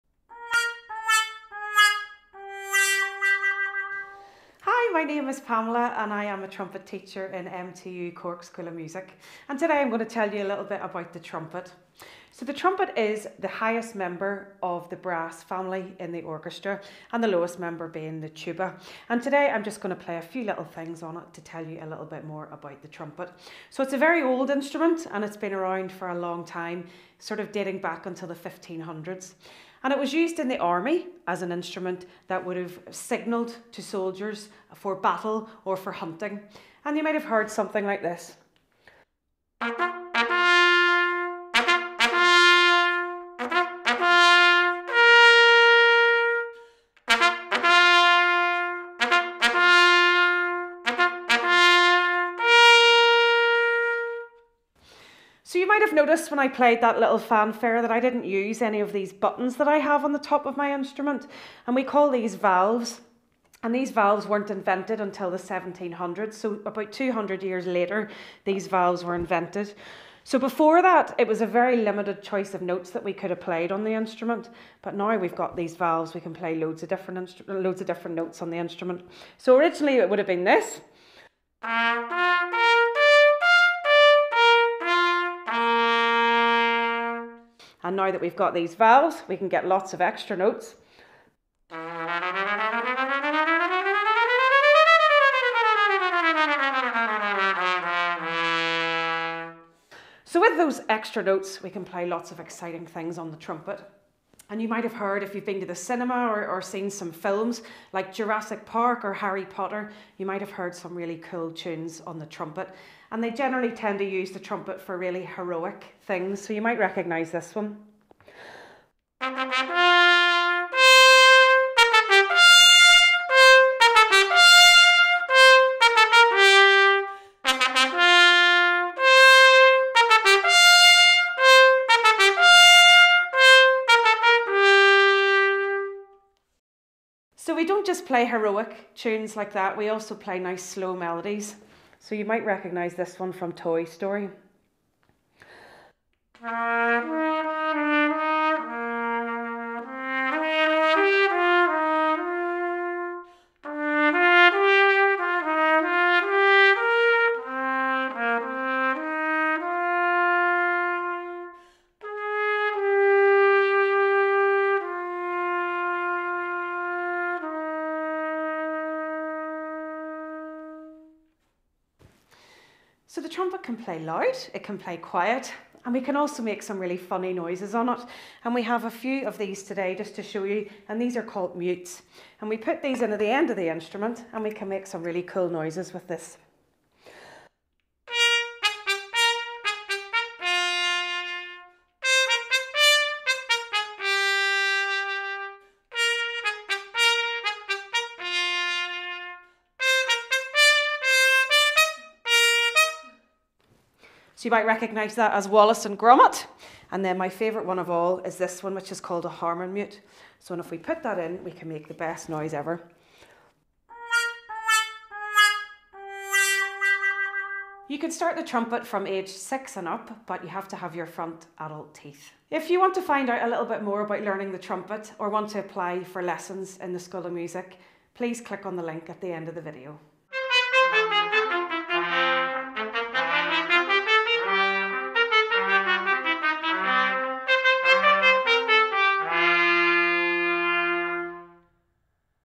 Choosing an Instrument: Trumpet with Pamela Stainer | MTU Cork School of Music
Lecturer Pamela Stainer introduces the Trumpet, a member of the brass family. Can you recognise some of the pieces that she plays? Students can start from the age of 6 upwards provided that they have their front adult teeth!